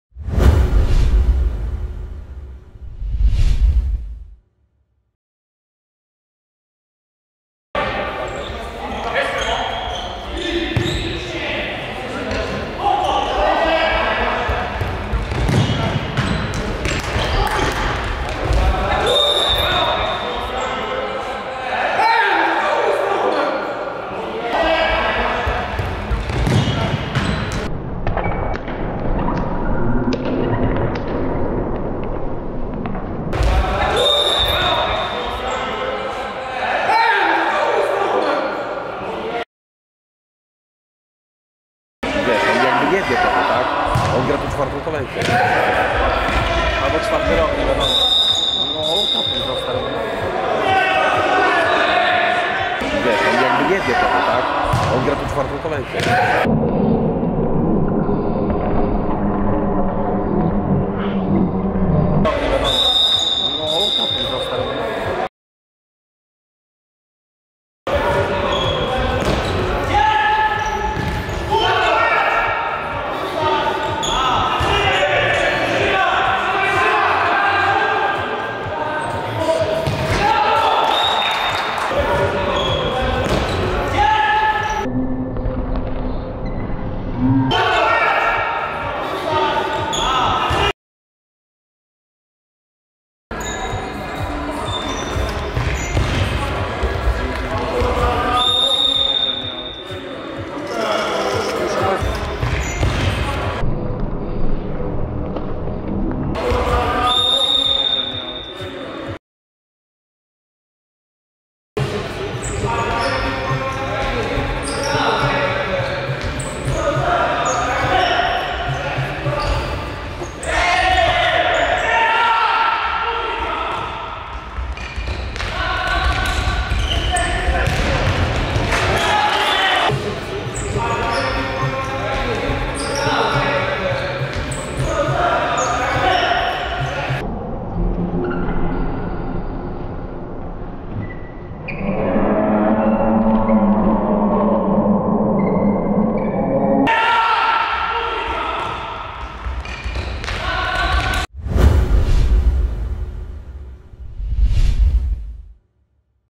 NLH: Kontrowersje - odcinek 4
Nocna Liga Halowa przedstawia najciekawsze sporne sytuacje z 4.kolejki trzynastej edycji. Czekamy na Wasze opinie na